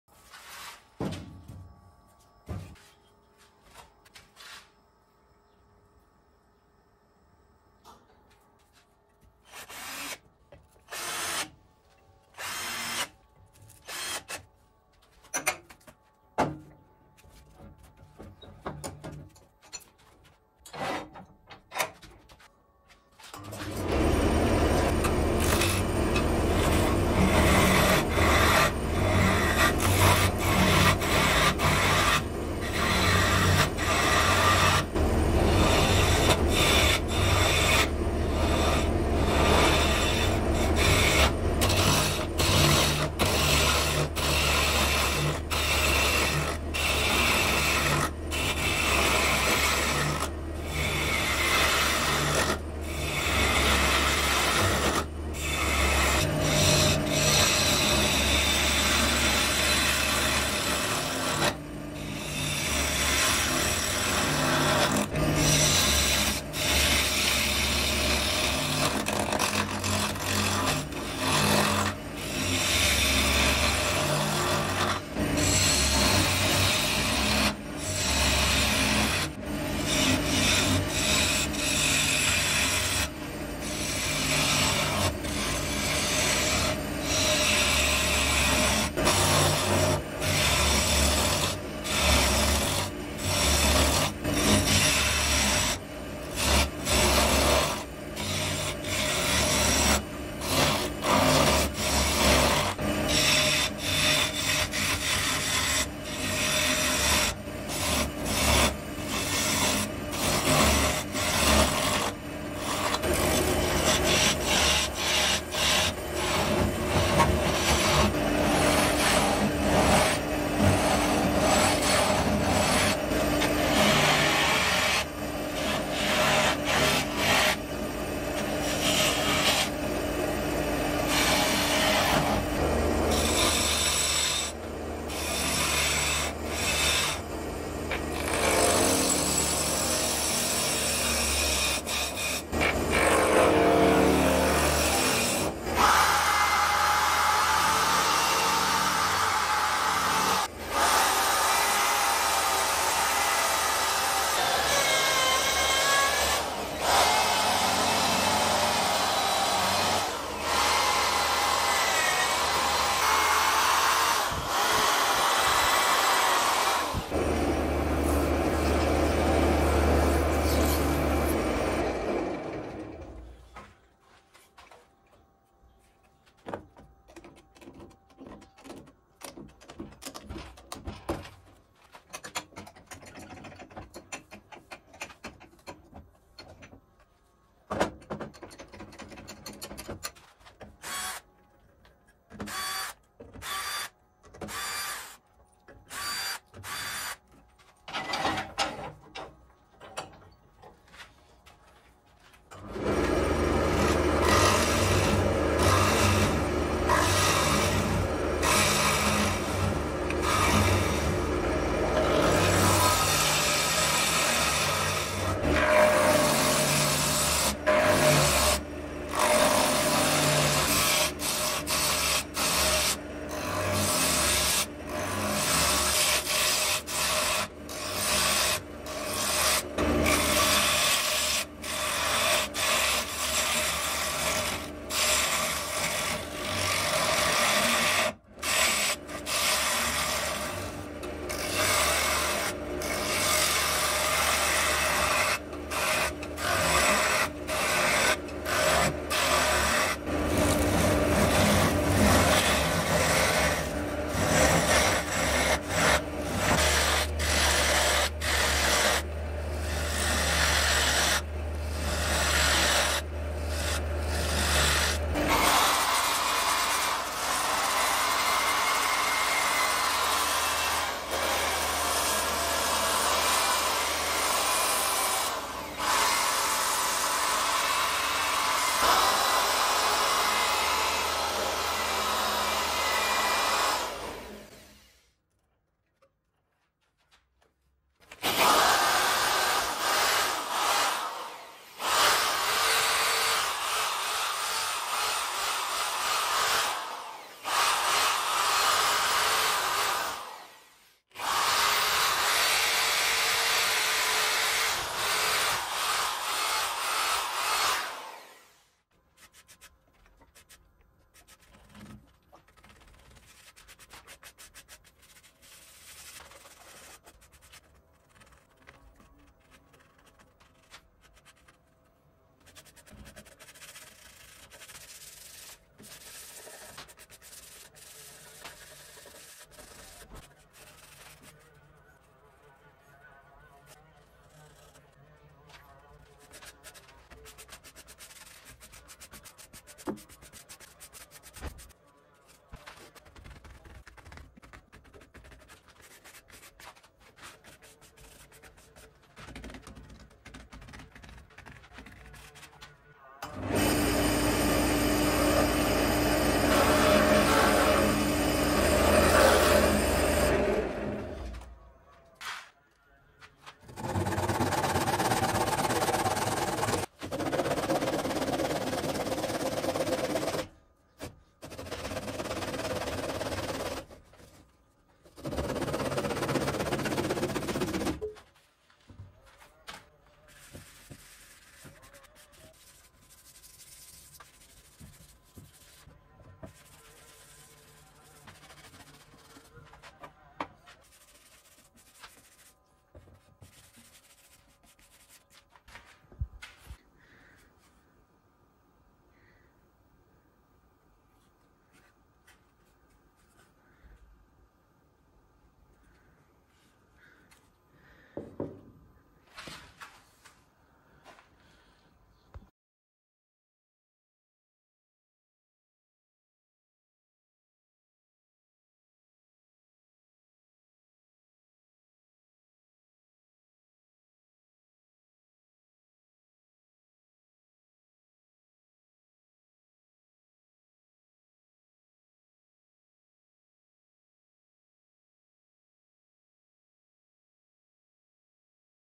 Woodturning - Fire Plate
Turning a log affected by the fungus into a plate of fire.

If you find my Woodturning videos entertaining don’t forget to share them with your likeminded friends.